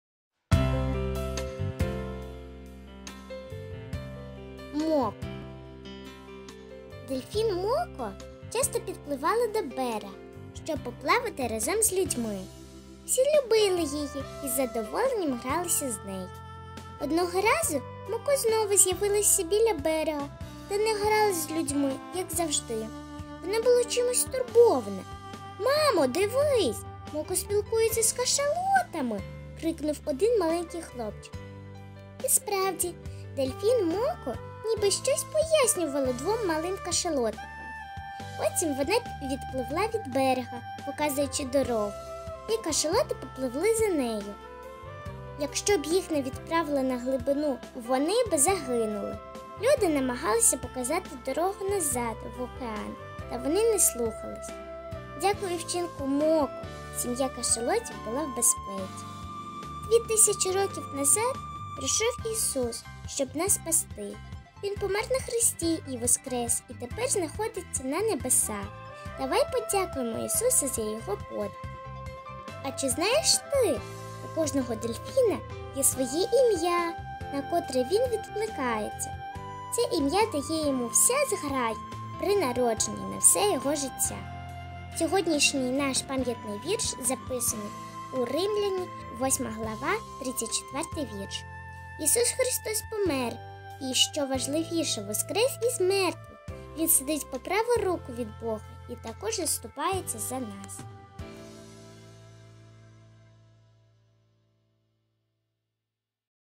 Моко / 7 липня
Дитячі ранкові читання.
Читаємо книгу Марини Становкіної "Усатые, пернатые и хвостатые".
Читає: Козлова Єва, м.Українка, Київська обл.